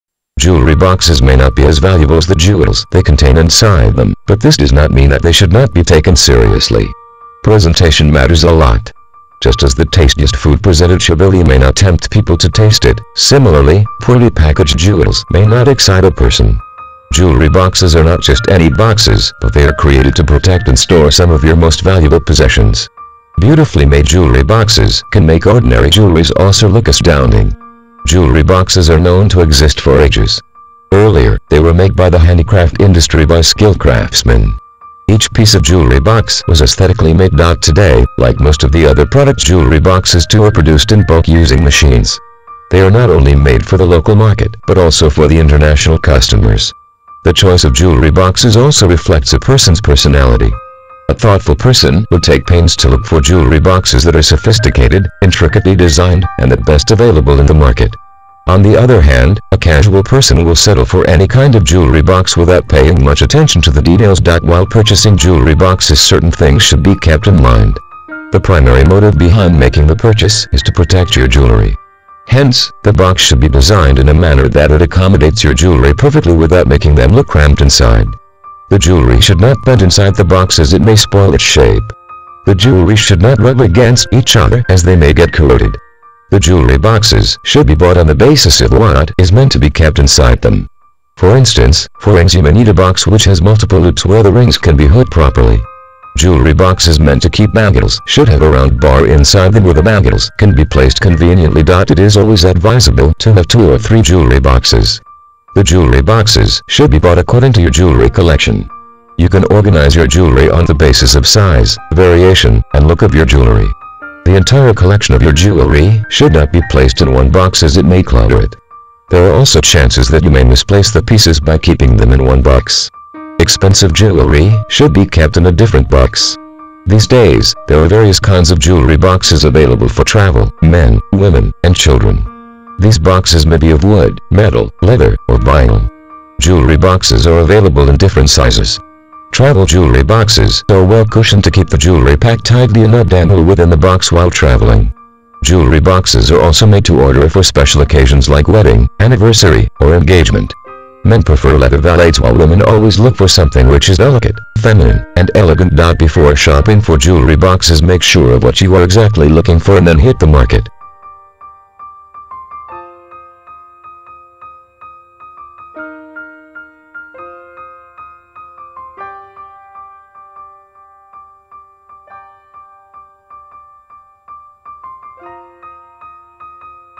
JewelryBoxes
Jewelry Boxes may not be as valuable as the jewels they contain inside them, but this does not mean that they should not be taken seriously. Presentation matters a lot. Just as the tastiest food presented shabbily may not tempt people to taste it, similarly, poorly packaged jewels may not excite a person.
Jewelry boxes are not just any boxes but they are created to protect and store some of your most valuable possessions. Beautifully made jewelry boxes can make ordinary jewelries also look astounding.  Jewelry boxes are known to exist for ages. Earlier, they were made by the handicraft industry by skilled craftsmen. Each piece of jewelry box was aesthetically made.
Today, like most of the other products jewelry boxes too are produced in bulk using machines. They are not only made for the local market but also for the international customers.  The choice of jewelry boxes also reflects a person's personality. A thoughtful person would take pains to look for jewelry boxes that are sophisticated, intricately designed, and the best available in the market.  On the other hand, a casual person will settle for any kind of jewelry box without paying much attention to the details.
While purchasing jewelry boxes certain things should be kept in mind. The primary motive behind making the purchase is to protect your jewelry. Hence, the box should be designed in a manner that it accommodates your jewelry perfectly without making them look cramped inside.  The jewelry should not bend inside the box as it may spoil its shape. The jewelries should not rub against each other as they may get corroded.
The jewelry boxes should be bought on the basis of what is meant to be kept inside them. For instance, for rings you may need a box which has multiple loops where the rings can be hooked properly.  Jewelry boxes meant to keep bangles should have a round bar inside them where the bangles can be placed conveniently.
It is always advisable to have two or three jewelry boxes. The jewelry boxes should be bought according to your jewelry collection. You can organize your jewelry on the basis of size, variation, and look of your jewelry. The entire collection of your jewelry should not be placed in one box as it may clutter it. There are also chances that you may misplace the pieces by keeping them in one box. Expensive jewelry should be kept in a different box.
These days, there are various kinds of jewelry boxes available for travel, men, women, and children. These boxes may be of wood, metal, leather, or vinyl. Jewelry boxes are available in different sizes. Travel jewelry boxes are well-cushioned to keep the jewelry packed tightly and not dangle within the box while traveling. Jewelry boxes are also made-to-order for special occasions like wedding, anniversary, or engagement.  Men prefer leather valets while women always look for something which is delicate, feminine, and elegant.
Before shopping for jewelry boxes make sure of what you are exactly looking for and then hit the market.